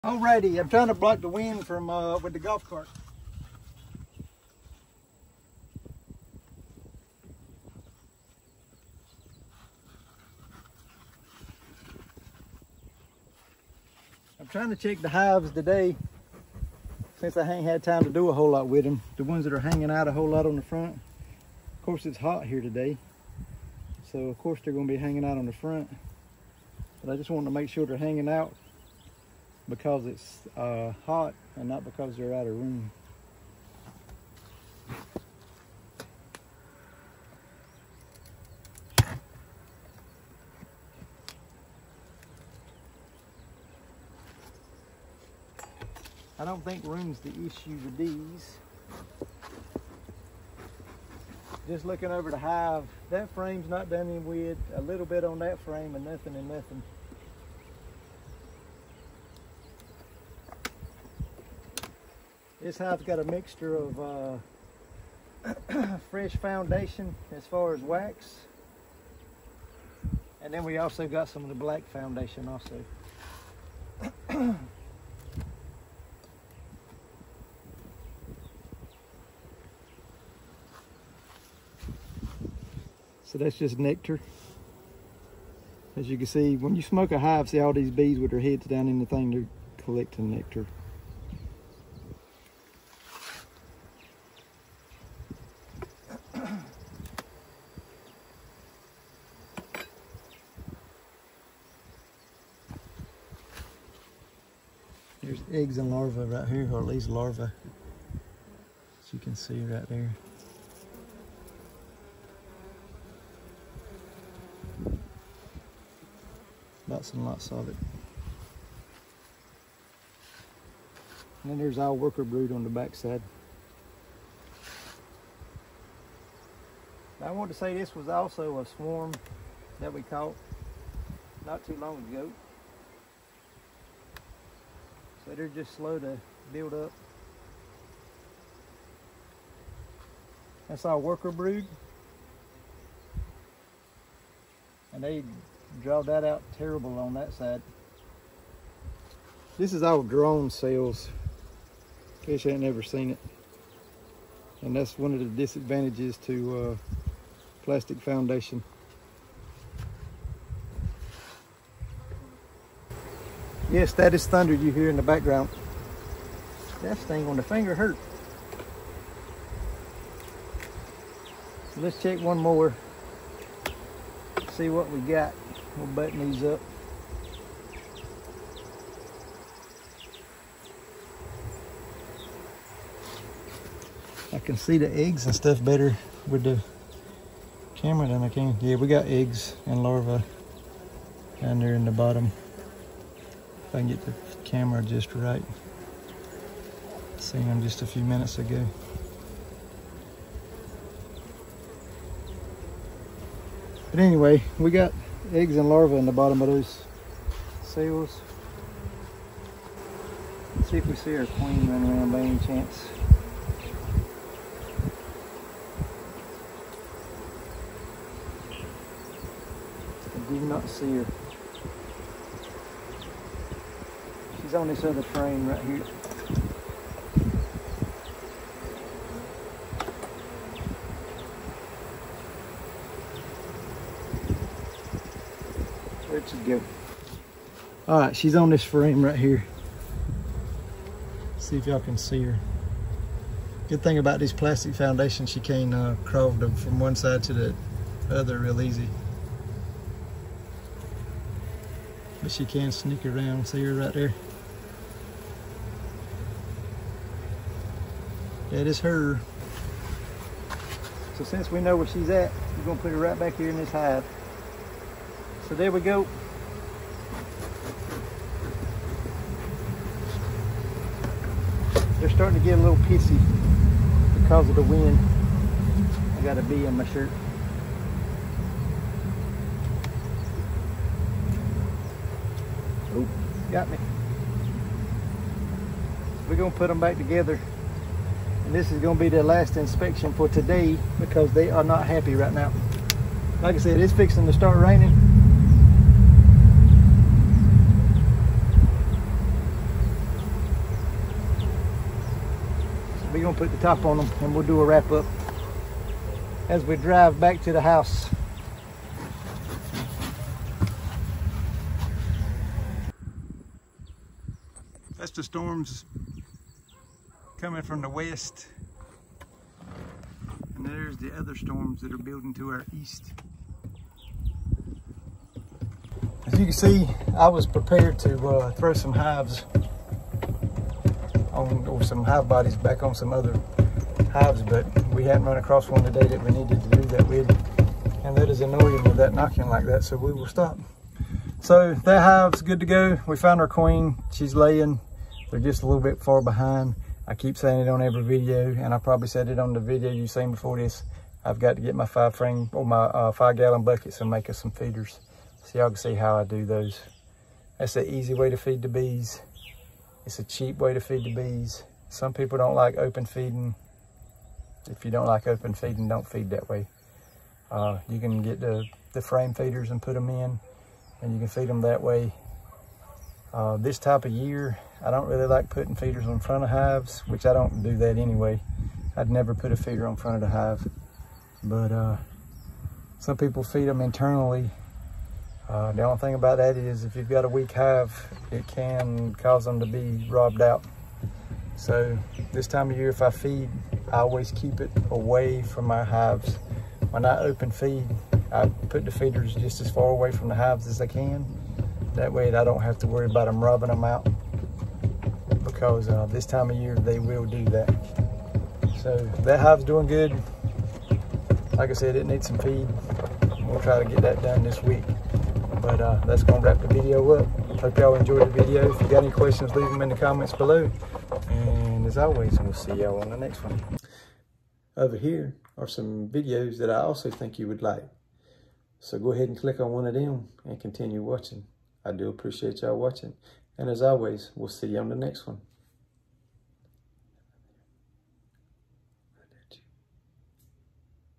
Find The Queen In A Thunderstorm. We Are Also Showing Close Up Shots Of Whats Inside The Hive
In this video we are inspecting this hive just before a thunderstorm hits. Bad weather equals mad bees. Weather has a very big effect on the honey bees act. We also show close ups of the eggs and larva inside the hive.

Honey extractor I have